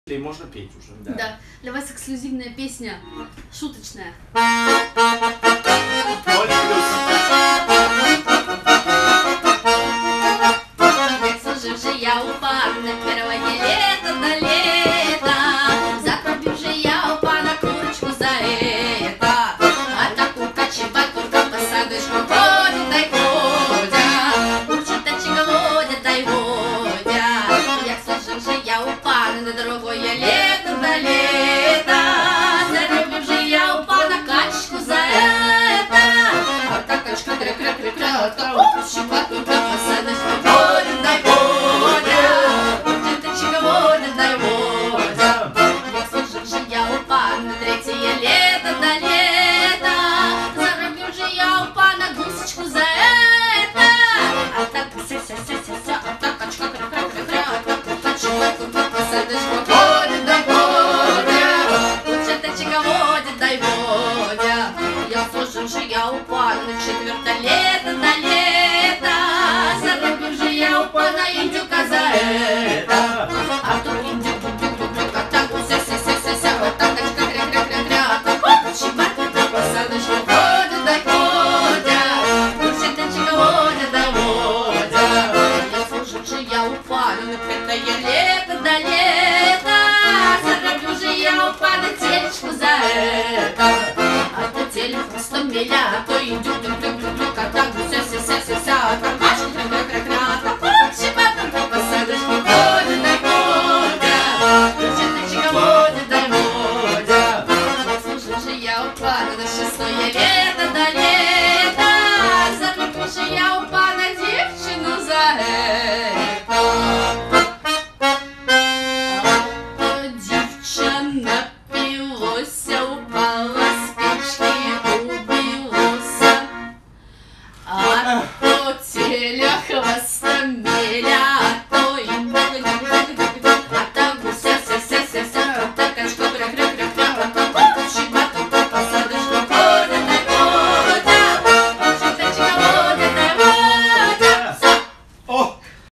Лия Брагина, Иван Разумов и Сергей Лебедев - Шуточная песня 🎶😁
Творческая встреча "У Вани на диване", г. Санкт-Петербург, 28 декабря 2018г. Лия Брагина Иван Разумов а также сайт с трансляциями "Прямых эфиров" , Сергей Лебедев